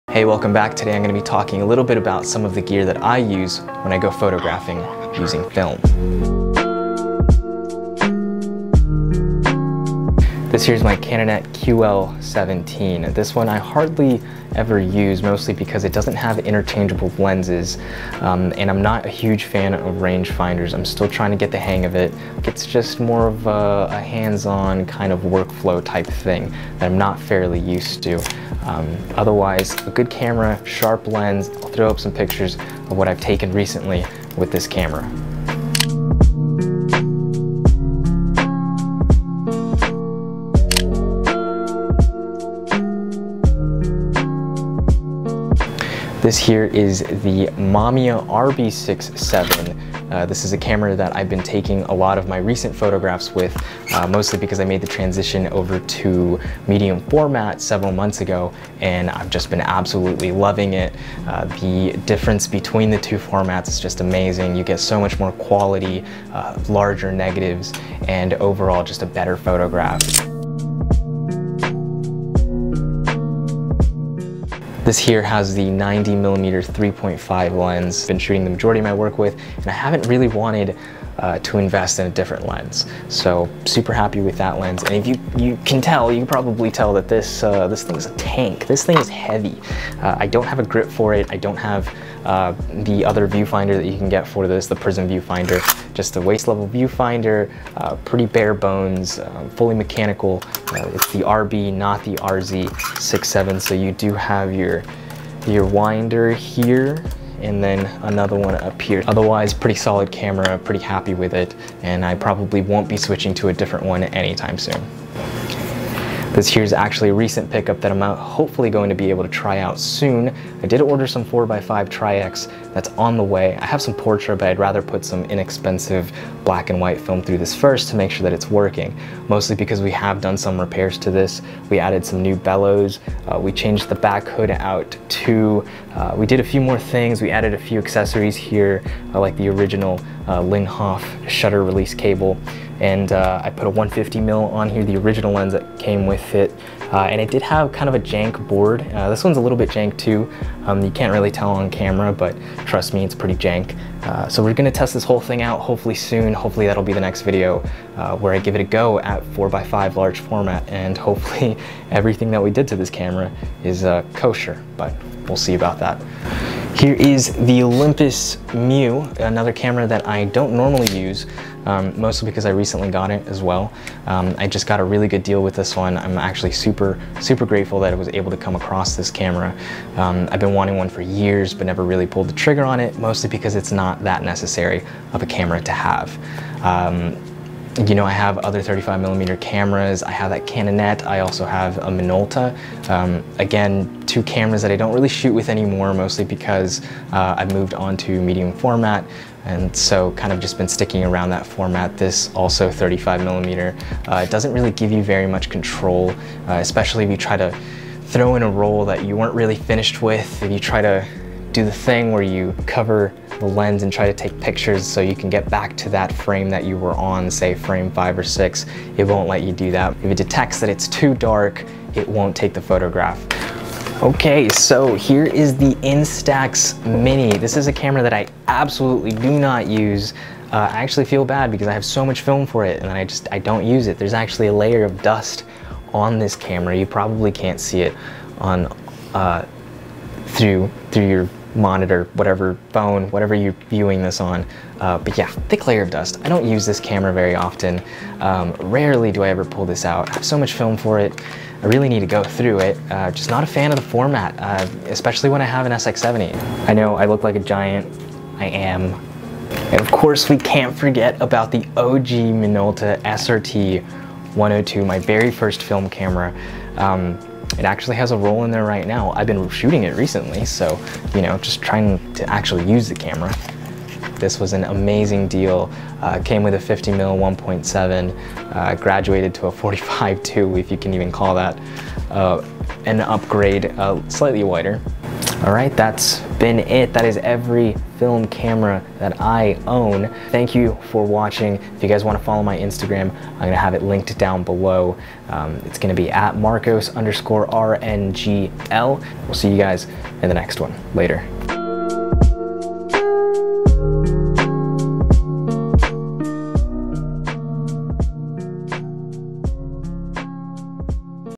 My Film Camera Collection
In this video, I talk about my camera collection and what I use and don't use on a regular basis.